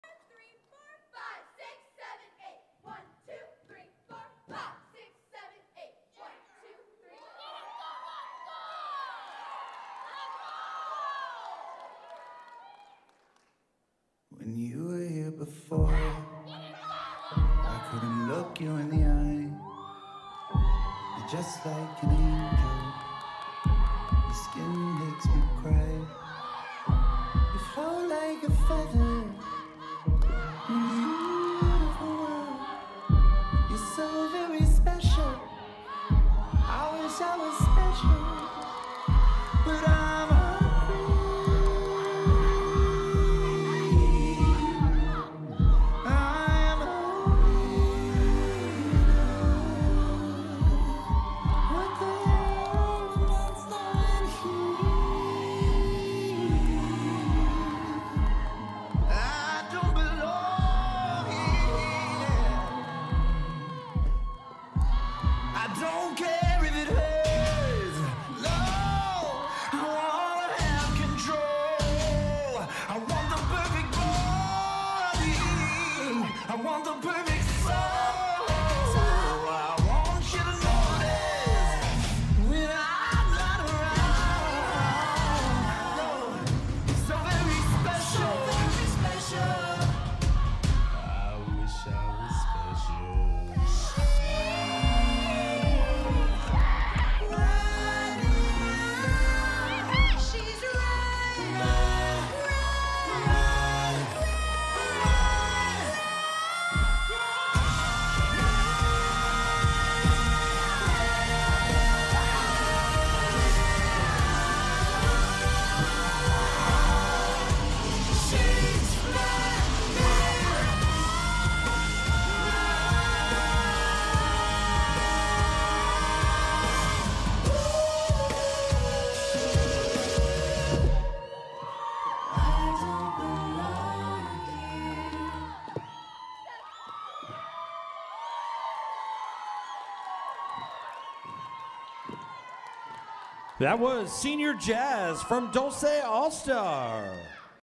Dolce Dance Studio - Senior Jazz 2024 [FINALS]
4th Place in Small Senior Jazz Division
The Dance Worlds - April 29, 2024
Dolce Dance Studio - Fresno, CA
Senior All-Star Team